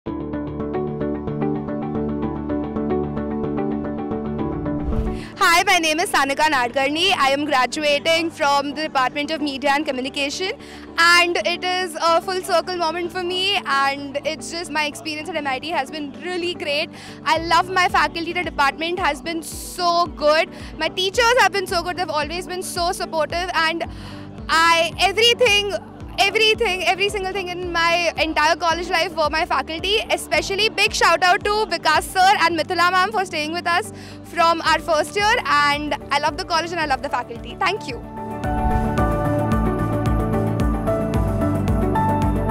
Sanika Nadkarni shares her experience at MIT-WPU
Sanika Nadkarni, a proud alumnus of the BA Media and Communication at MIT World Peace University, reflects on her enriching college experience. She emphasizes the exceptional support provided by the faculty throughout her academic journey, noting their commitment to assisting students with both the curriculum and practical opportunities. This nurturing environment not only facilitated her learning but also played a crucial role in her professional development, preparing her for a successful career in media.